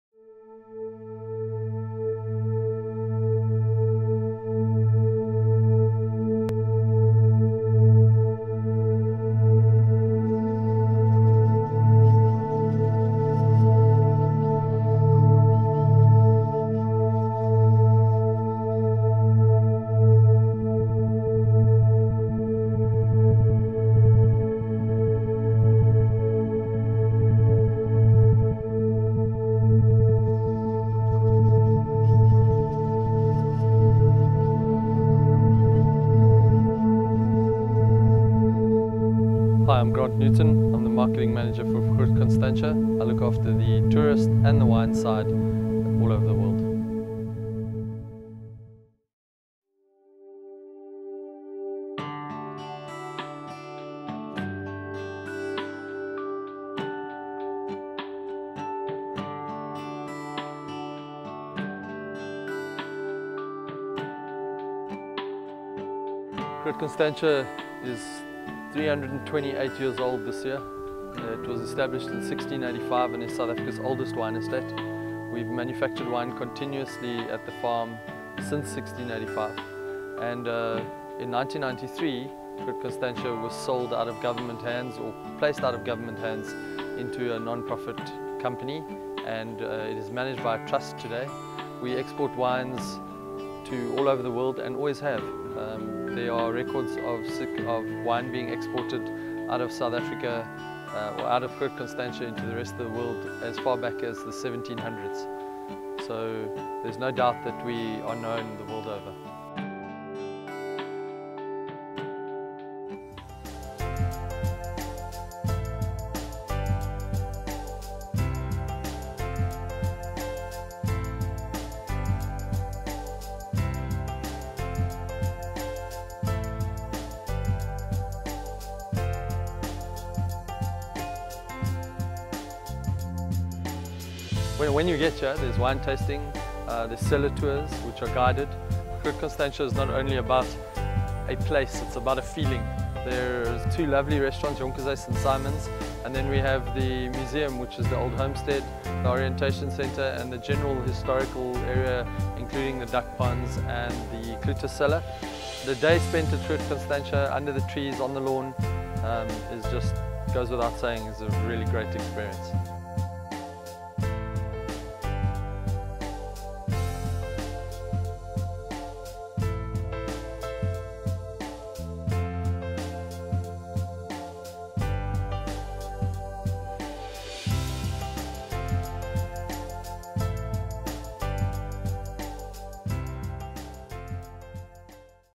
Groot Constantia Wine Estate [Cape Town Tourism]
A Promotional video showcasing the lovely Groot Constantia Wine Estate
situated in Cape Town, South Africa.